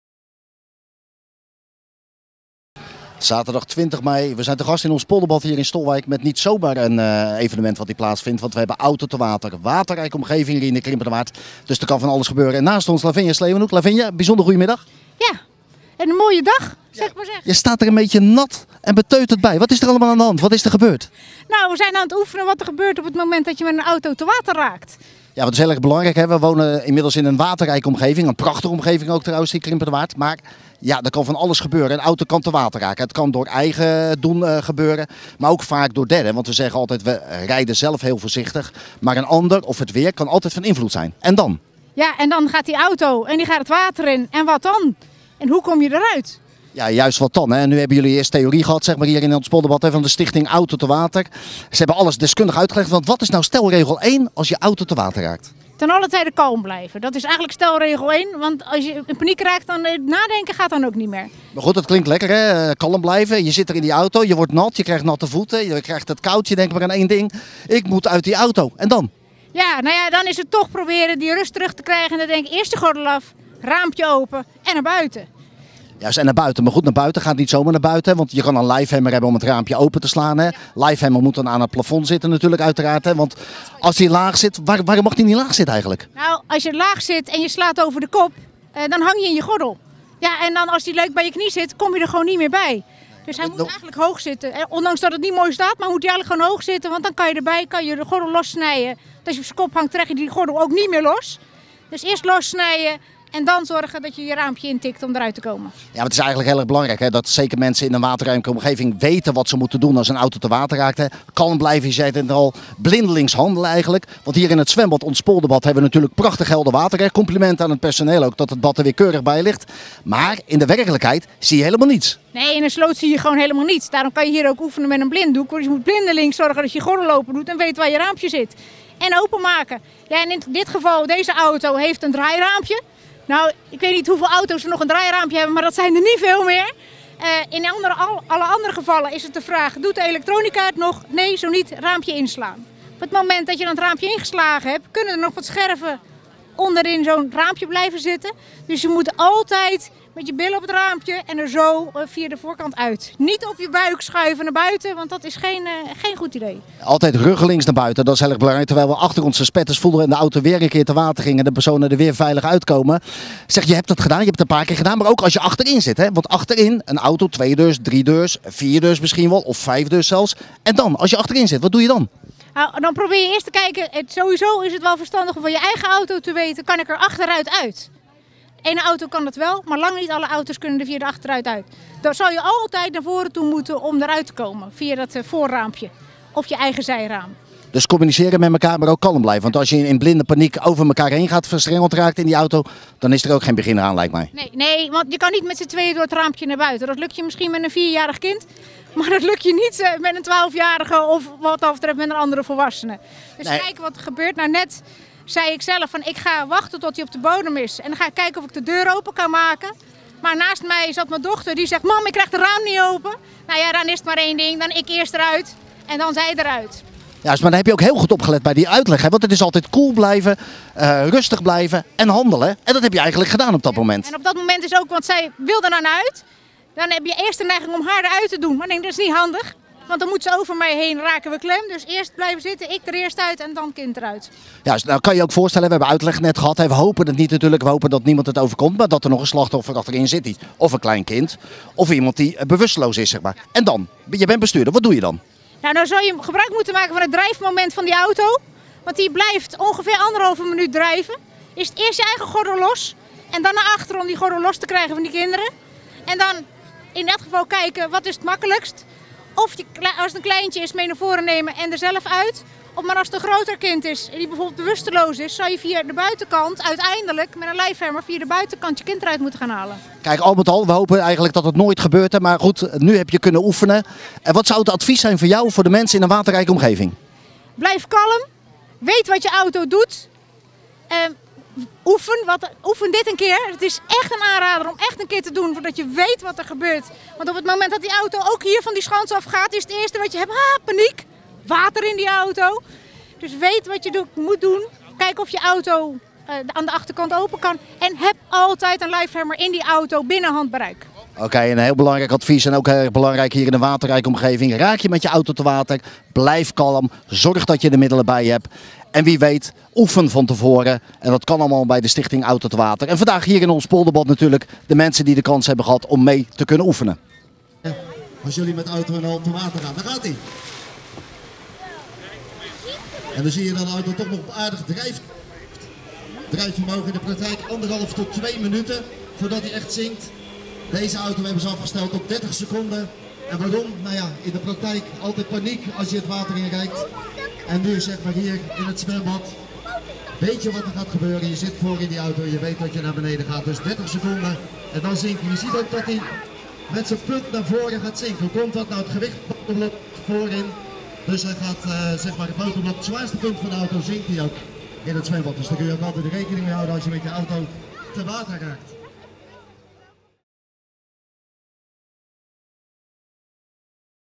Cursus Auto te water in Ons Polderbad
Zaterdag 20 mei stond Ons Polderbad in Stolwijk volledig in het teken van de cursus Auto te water. Verschillende deelnemers waaronder wethouder Lavinja Sleeuwenhoek namen de uitdaging aan en gingen, na een theoriegedeelte, met de auto te water.